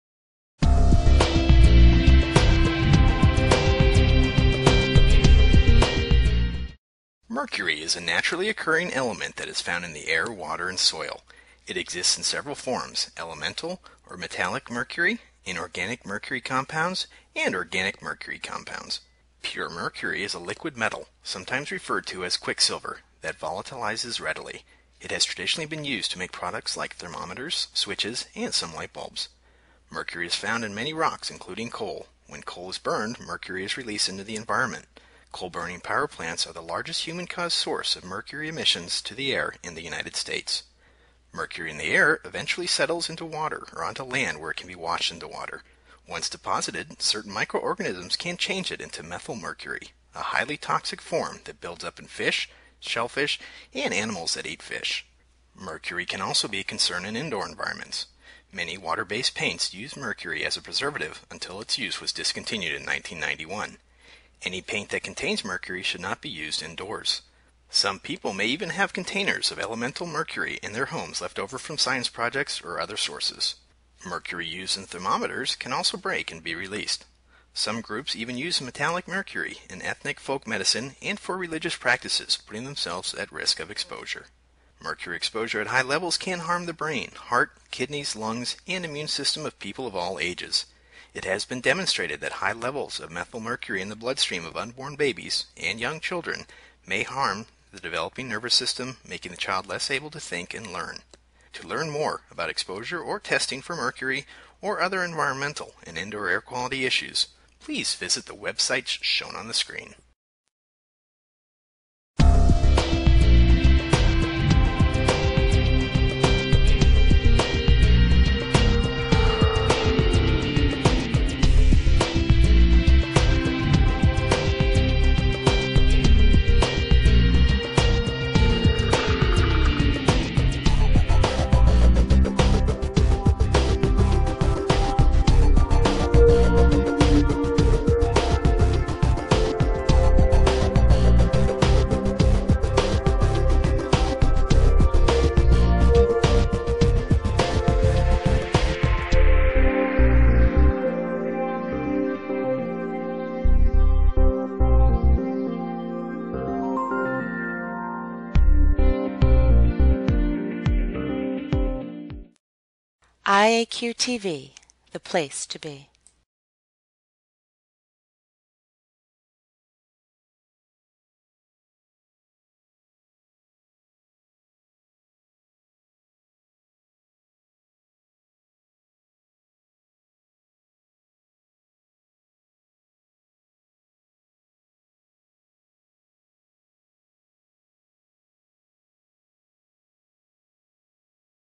Mercury Exposure and Health Effects
Mercury is a naturally occurring element that is found in air, water and soil. It exists in several forms: elemental or metallic mercury, inorganic mercury compounds, and organic mercury compounds.

Mercury exposure at high levels can harm the brain, heart, kidneys, lungs, and immune system of people of all ages. It has been demonstrated that high levels of methylmercury in the bloodstream of unborn babies and young children may harm the developing nervous system, making the child less able to think and learn.